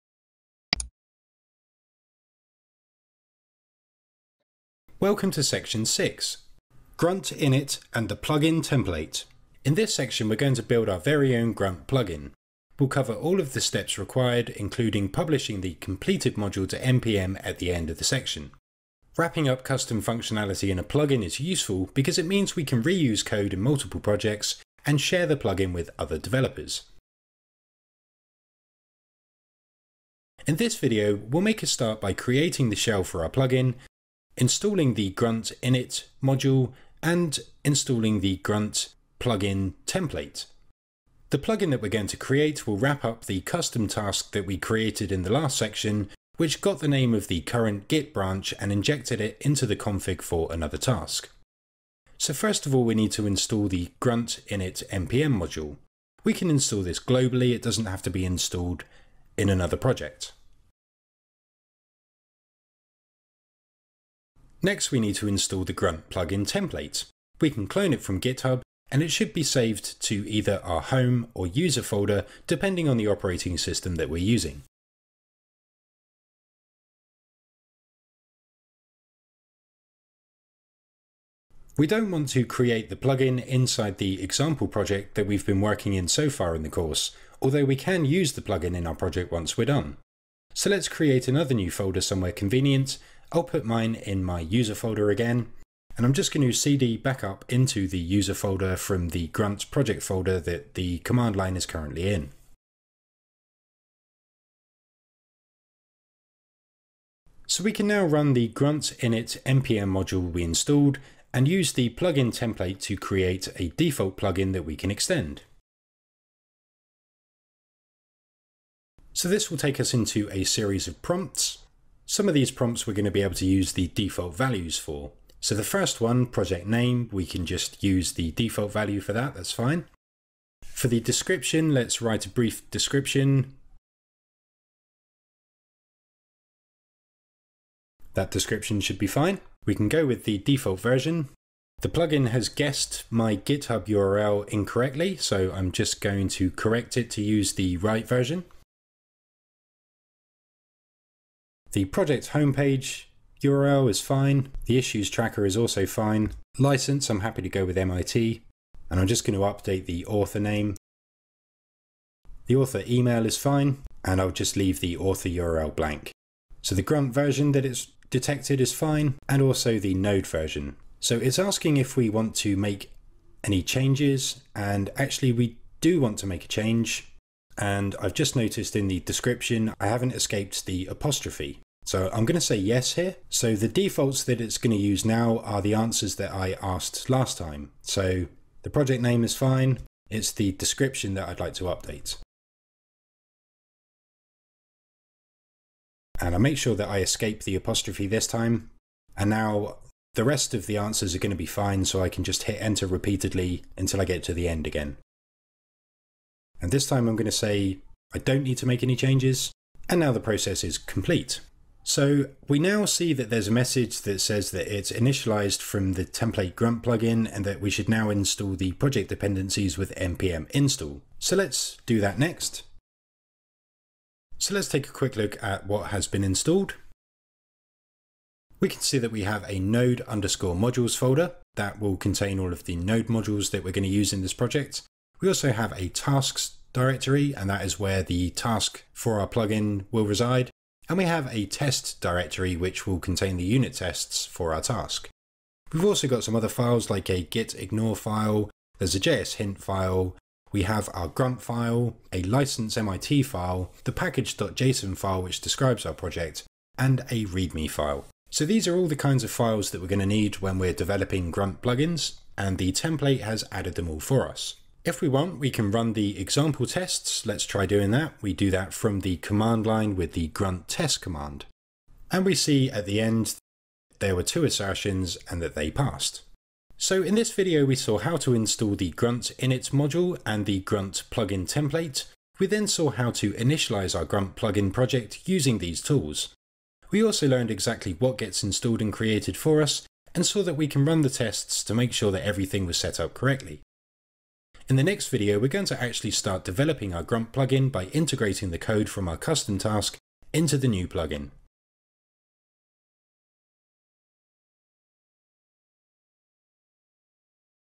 Learning Grunt: Grunt init and the Plugin Template | packtpub.com
We can use the Grunt init plugin and the plugin template to quickly get started when creating a new plugin.
• Install the Grunt-init plugin
• Install the plugin template
• Initialize a new plugin